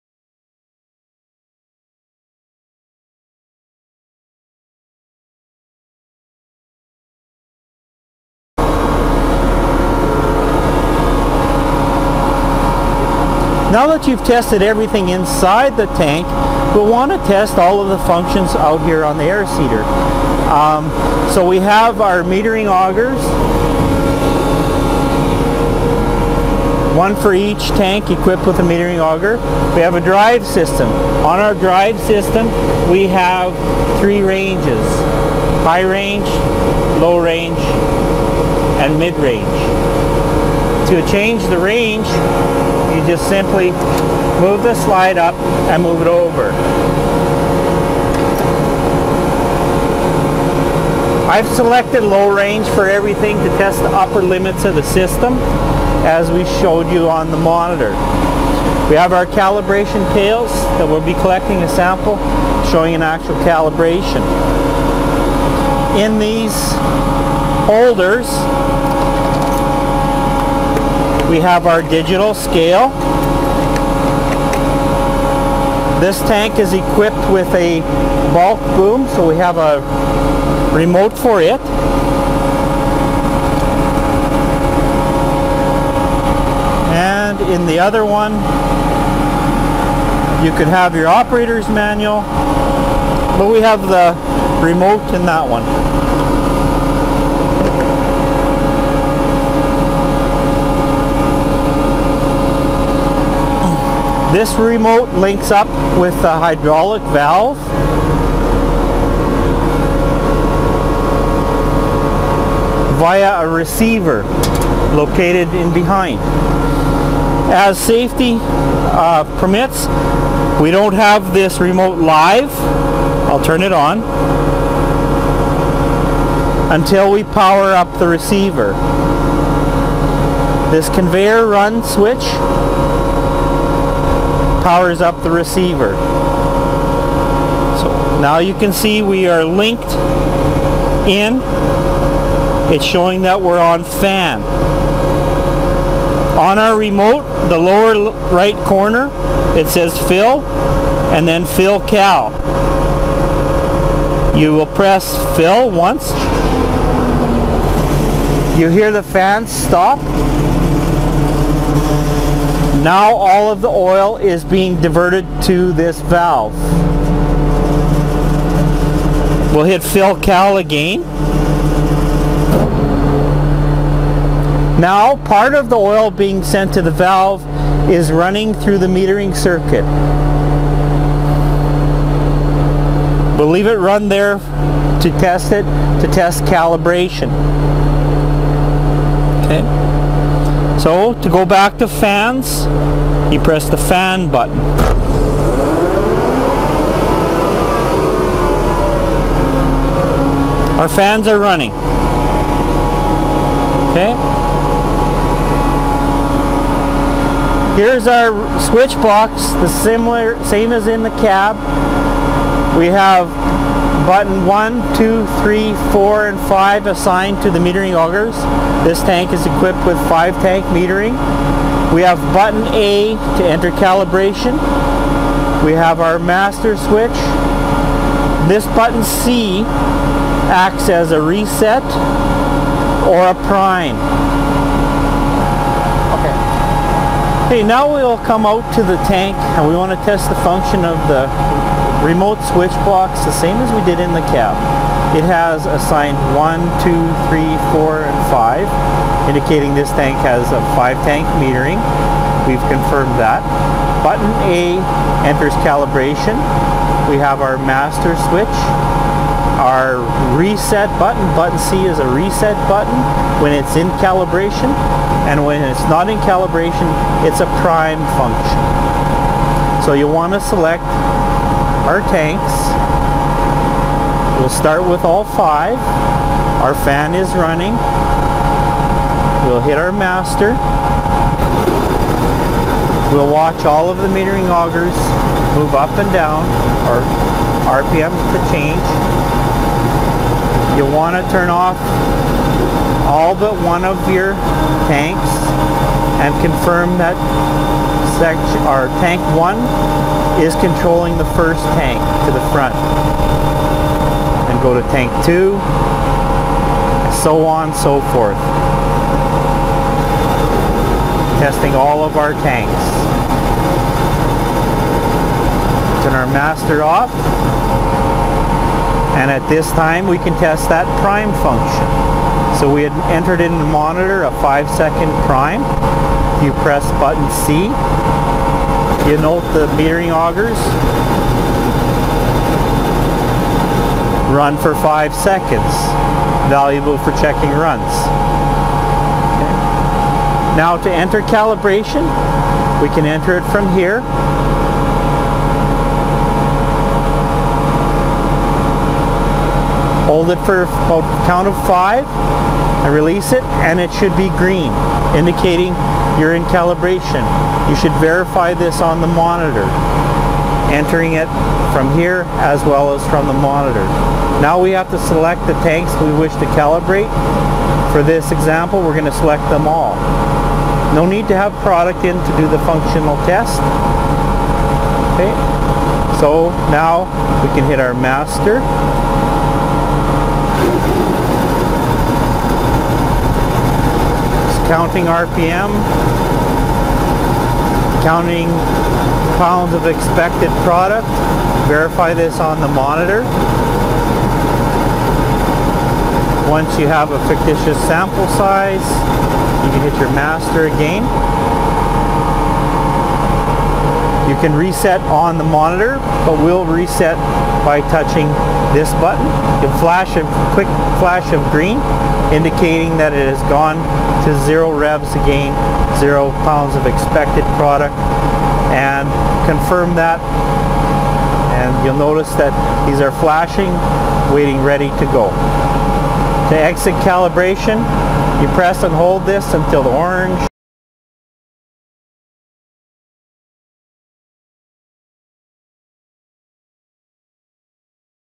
Chapter 4 - Bourgault 7000 Series w/o ASC Function Test
This video is the fourth of five which covers functional testing of a Bourgault 7000 Series Air Seeder not equipped with Auto Sectional Control. It is important to perform these tests prior to first use, and before seeding each following year.

Chapter 1 - Getting Started
Chapter 2 - Monitor Settings
Chapter 3 - Monitor Testing
Chapter 4 - On Tank Testing
Chapter 5 - Conveyor/Auger and Weigh Scales